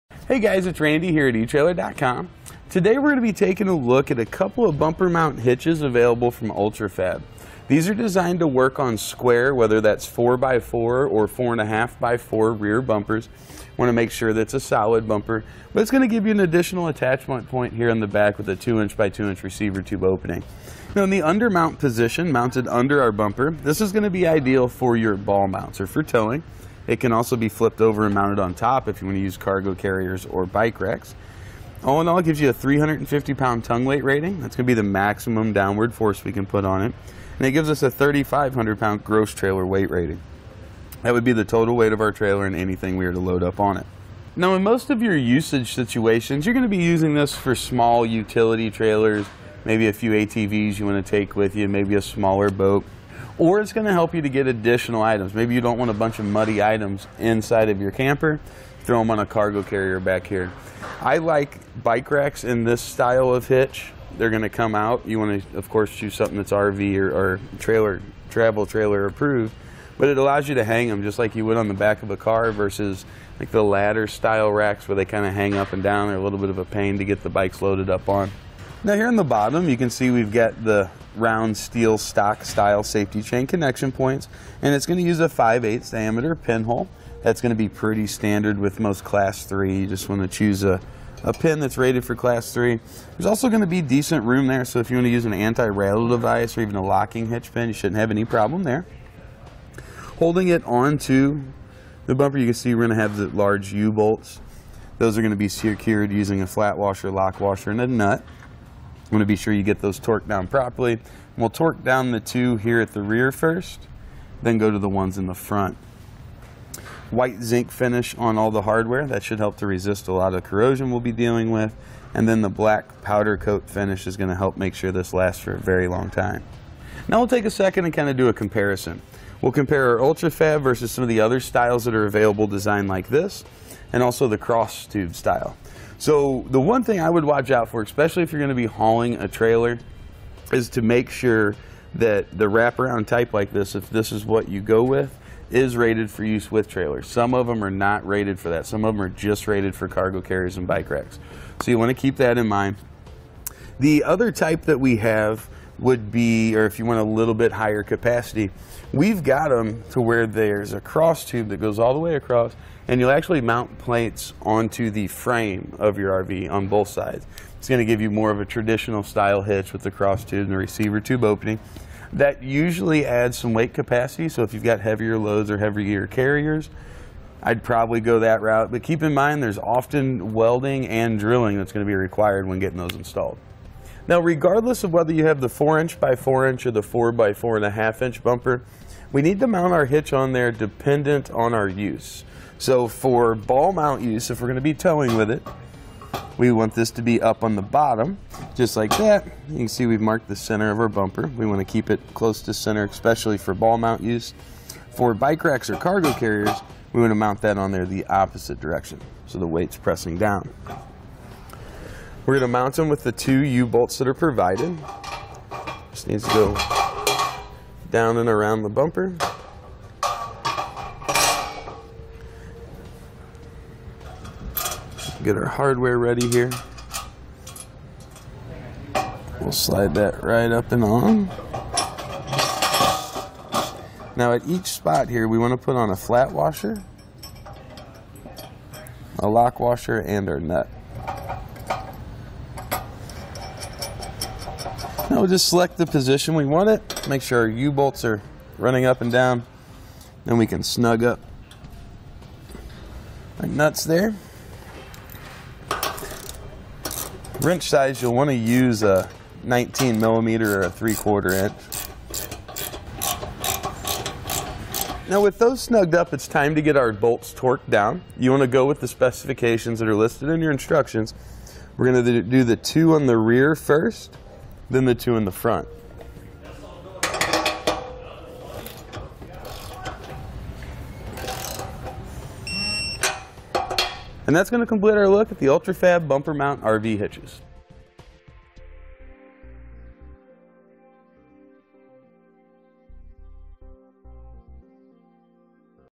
etrailer | Ultra-Fab 2" Motor Home Trailer Hitch Receiver for 4" x 4" Bumpers: The Breakdown
Click for more info and reviews of this Ultra-Fab Products RV and Camper Hitch:

Randy:              Hey guys, it's Randy here at etrailer.com. Today we're going to be taking a look at a couple of bumper mount hitches available from Ultrafab. These are designed to work on square, whether that's four by four, or four and a half by four rear bumpers. Want to make sure that it's a solid bumper. But it's going to give you an additional attachment point here on the back, with a two inch by two inch receiver tube opening.Now in the under mount position, mounted under our bumper, this is going to be ideal for your ball mounts, they're for towing. It can also be flipped over and mounted on top, if you want to use cargo carriers or bike racks.

All in all, it gives you a 350 pound tongue weight rating. That's going to be the maximum downward force we can put on it. It gives us a 3,500 pound gross trailer weight rating. That would be the total weight of our trailer, and anything we we're to load up on it.Now in most of your usage situations, you're going to be using this for small utility trailers, maybe a few ATVs you want to take with you, maybe a smaller boat. Or, it's going to help you to get additional items.

Maybe you don't want a bunch of muddy items inside of your camper, throw them on a cargo carrier back here.I like bike racks in this style of hitch. They're going to come out, you want to of course choose something that's RV or travel trailer approved, but it allows you to hang them just like you would on the back of a car versus the ladder style racks where they kind of hang up and down, are a little bit of a pain to get the bikes loaded up on.Now here in the bottom, you can see we've got the round steel stock style safety train connection points, and it's going to use a 5/8 diameter pinhole. That's going to be pretty standard with most class three, you just want to choose a pin that's rated for class three. There's also going to be decent room mass, so if you want to use an anti-rattle device, or even a locking hitch pin, you shouldn't have any problem there. Holding it onto the bumper, you can see we're going to have the large U bolts.

Those are going to be secured using a flat washer, lock washer and a nut. We want to be sure you get those torqued down properly. We'll torque down the two here at the rear first, then go to the ones in the front.White zinc finish on all the hardware. That should help to resist a lot of corrosion we'll be dealing with, and then the black powder coat finish is going to help make sure this lasts for a very long time.Now we'll take a second and kind of do a comparison. We'll compare our Ultrafab versus some of the other styles that are available design like this, and also the cross tube style.

The one thing I would watch out for, especially if you're going to be hauling a trailer, is to make sure that the wrap around type like this, if this is what you go with, Is rated for use with trailers. Some of them are not rated for that. Some of them are just rated for cargo carriers and bike racks. You want to keep that in mind.The other type that we have would be . Or, if you want a little bit higher capacity, we've got them to where there's a cross tube that goes all the way across, and you'll actually mount plates onto the frame of your RV, on both sides. It's going to give you more of a traditional style hitch, with a cross tube and the receiver tube opening. That usually adds some weight capacity, so if you've got heavier loads, or heavier carriers, I'd probably go that route, but keep in mind there's often welding and drilling that's going to be required when getting those installed.Now, regardless of whether you have the 4" by 4", or the 4" x 4.5" bumper, we need to mount our hitch on there dependent on our use. For ball mount use, if we're going to be towing with it, we want this to be up on the bottom, just like that. You can see we've marked the center of our bumper. We want to keep it close to the center, especially for ball mount use. For bike racks or cargo carriers, we want to mount that on there the opposite direction. So, the weight's pressing down.We're going to mount them with the two U-Bolts that are provided. This needs to go down and around the bumper. We'll get our hardware ready here. We'll slide that right up and on. Now at each spot here, we want to put on a flat washer, a lock washer and a nut. Now, we'll just select the position. We want to make sure our U-bolts are running up and down, then we can snug up my nuts there. Wrench size y